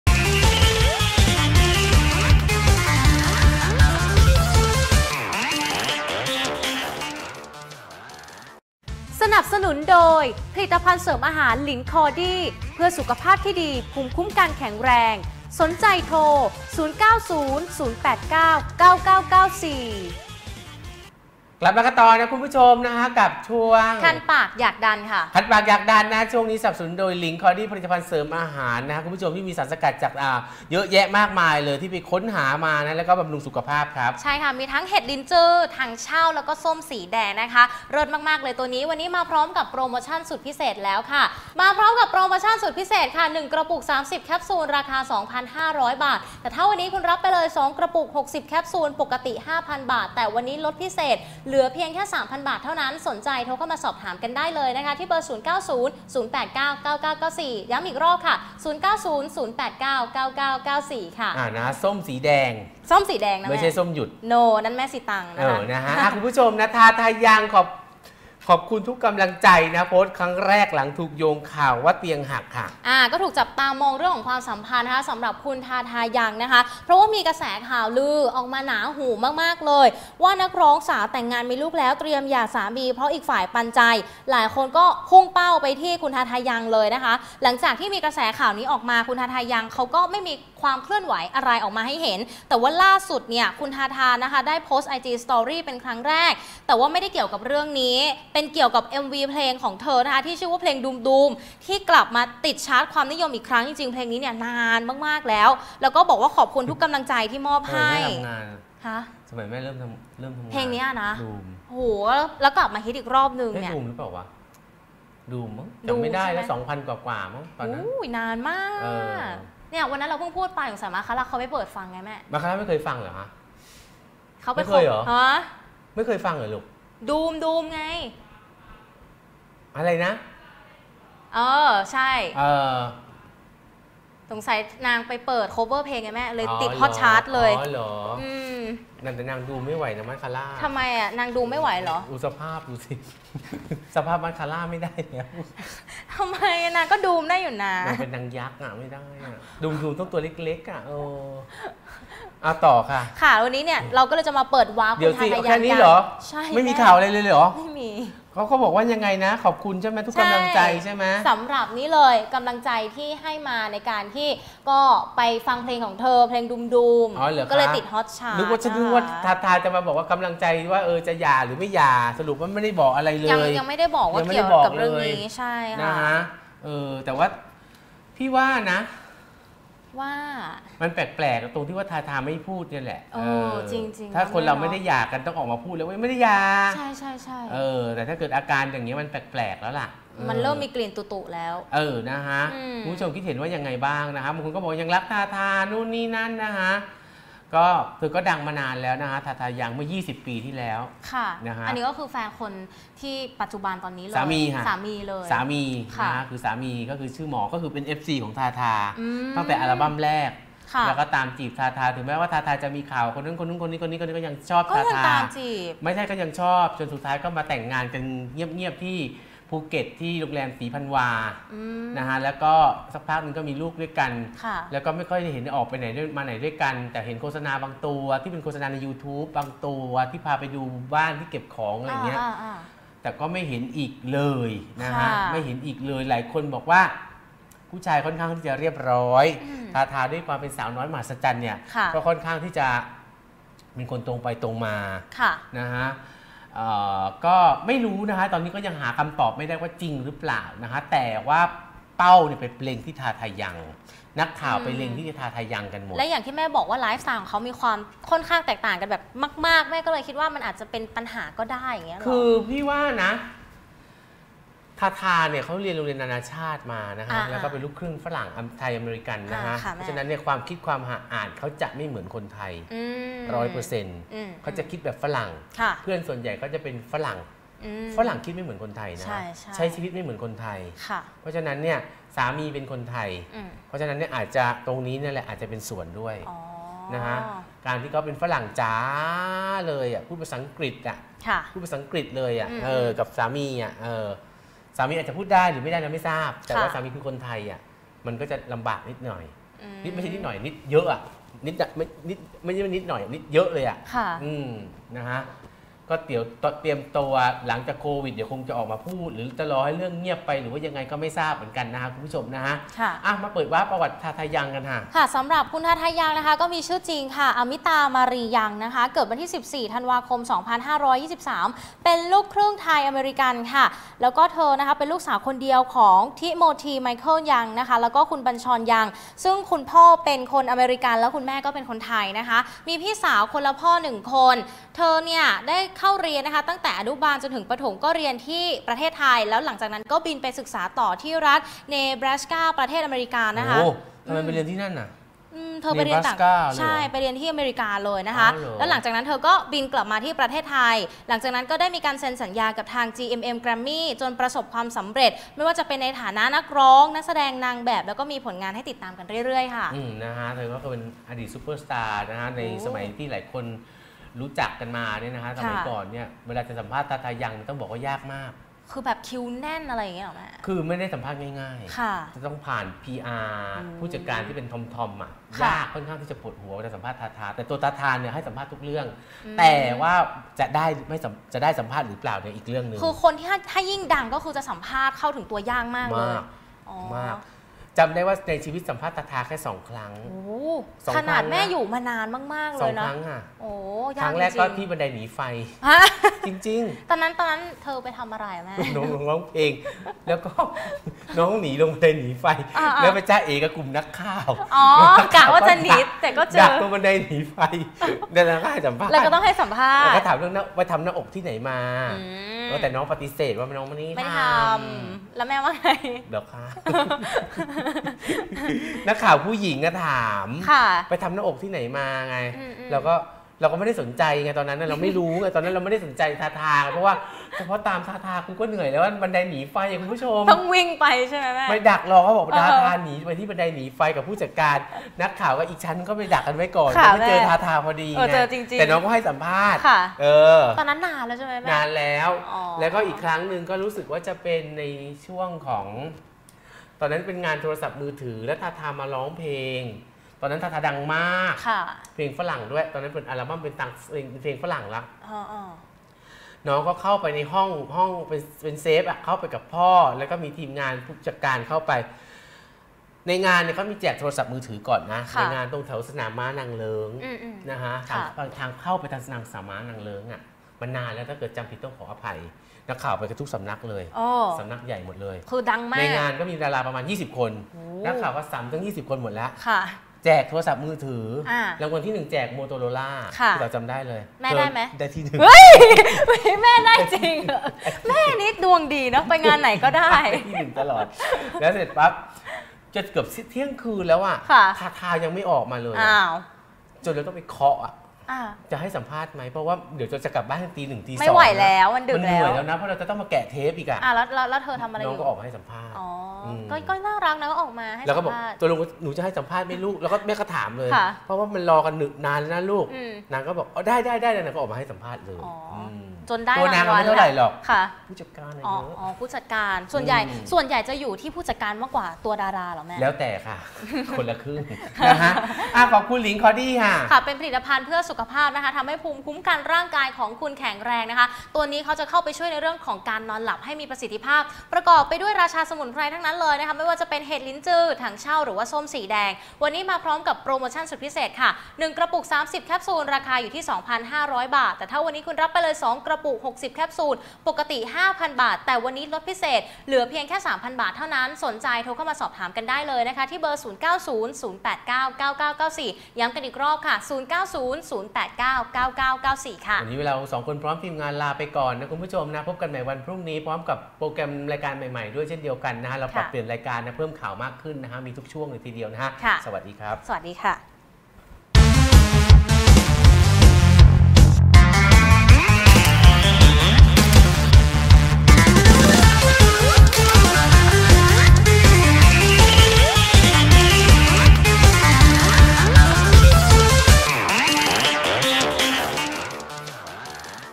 "ทาทา ยัง" ขอบคุณทุกกำลังใจ โพสต์ครั้งแรกหลังถูกโยงข่าว นักร้องเตรียมหย่าสามี
Facebook  เม้าท์มันส์คันปาก - Smarttv 61